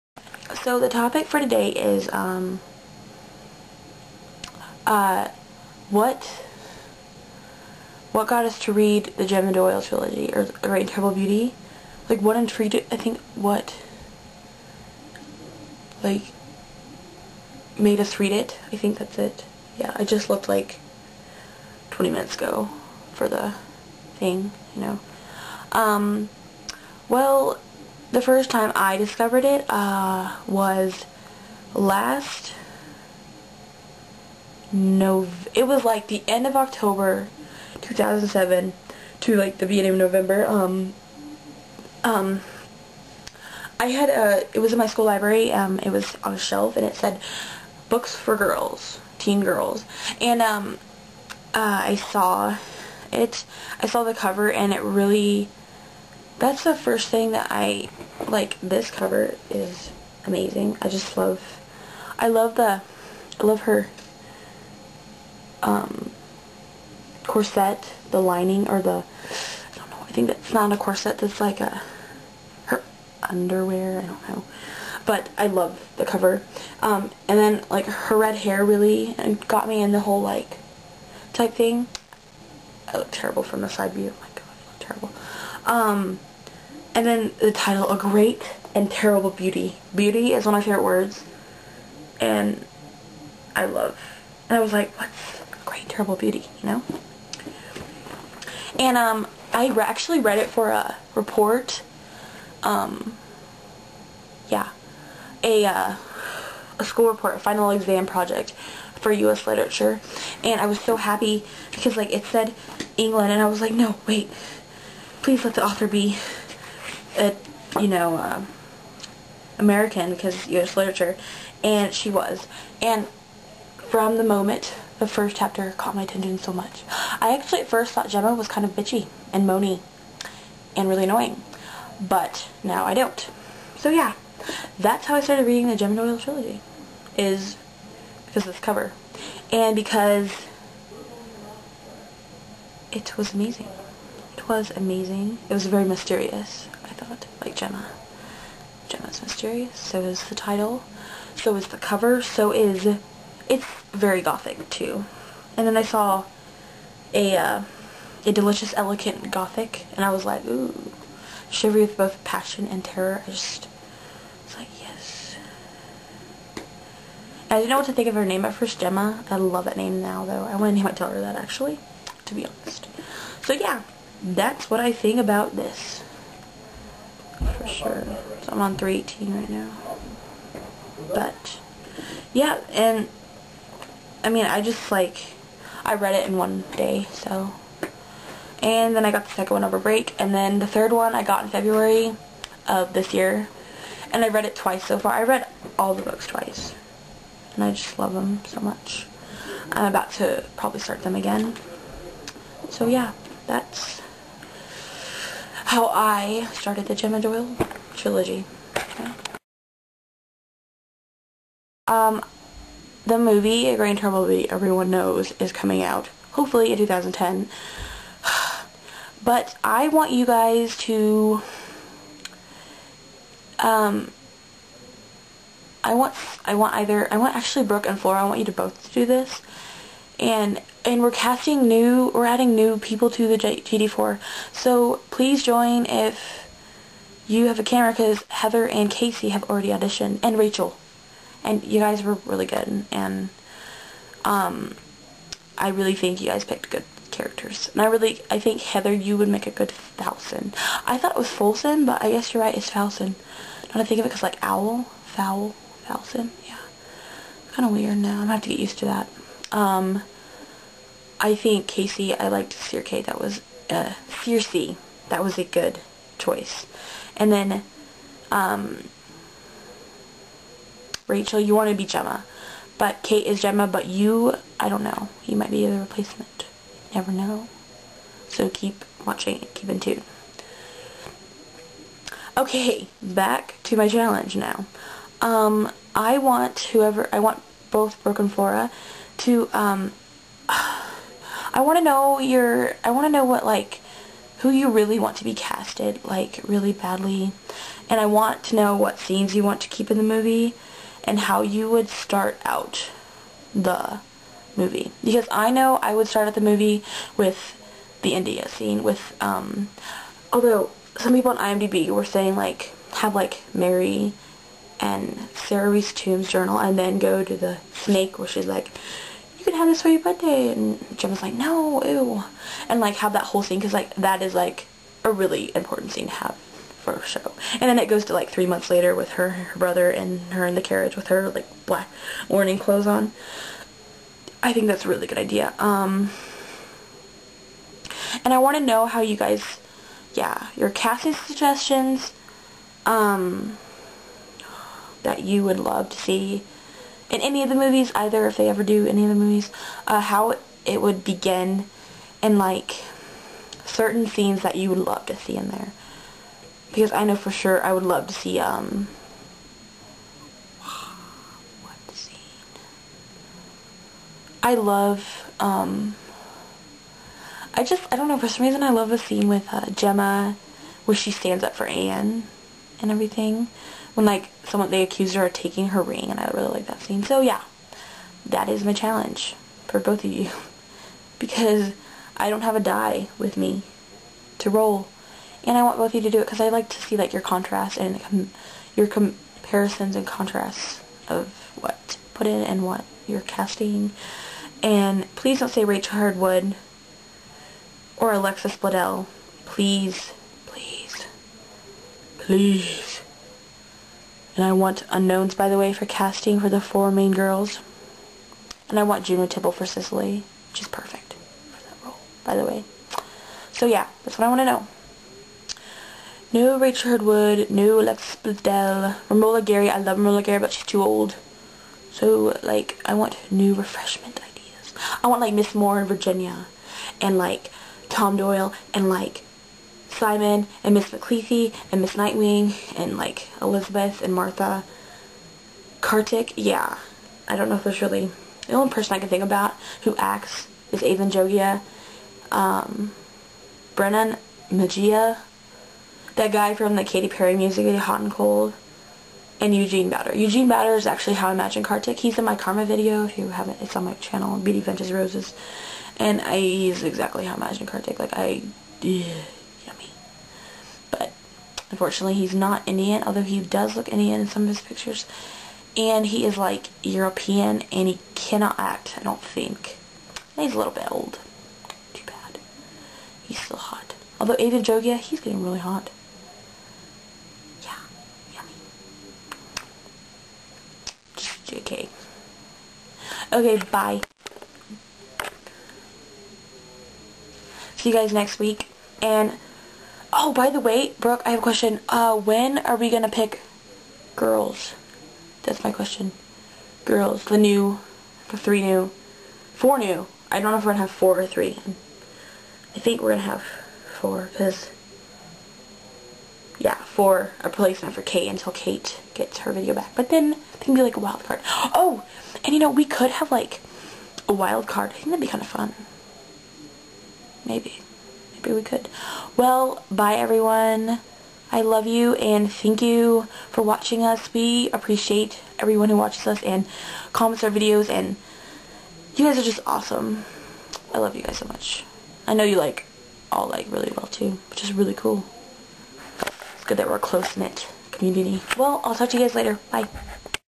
Ann on how she first discovered this EPIC trilogy
=) my discovery of AGATB, RA, and TSFT